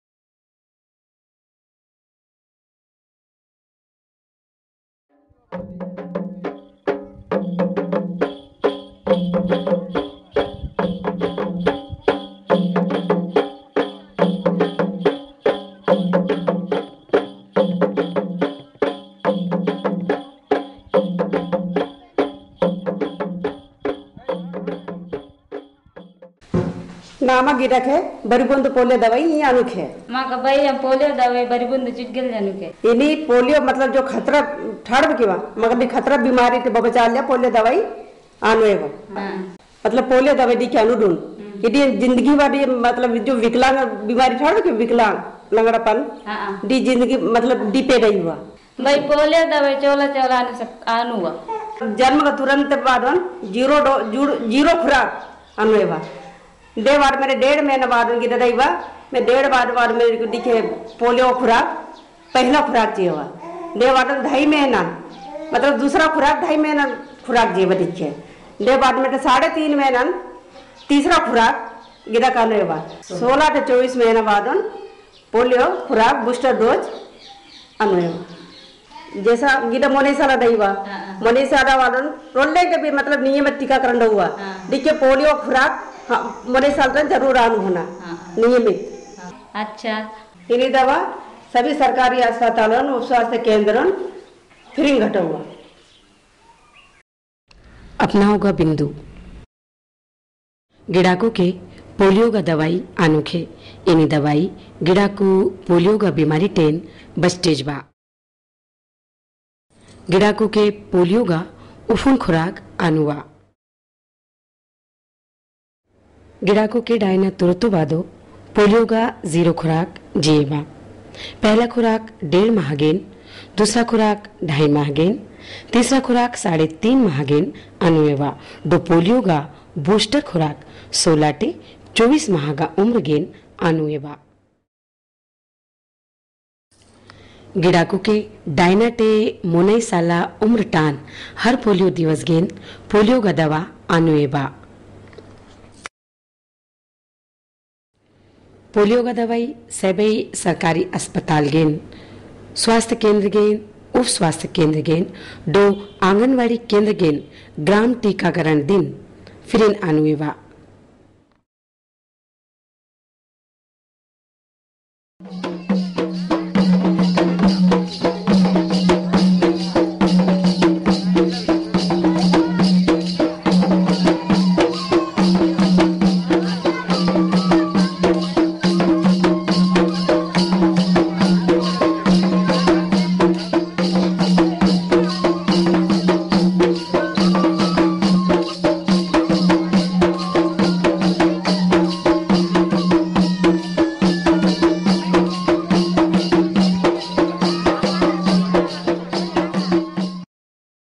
Importance of Polio vaccine_(Korku)_RMNT & Digital Green_Khandwa (M.P.)
This video provides basic information about oral polio vaccinations.
Oral Polio vaccine prevent child from polio infections, total Oral Polio vaccine dose provided to child through routine immunization. Oral Polio dose provided free of cost by all Govt. health centers and Aaganwadi center during VHND/ immunization day. For more information and related videos visit us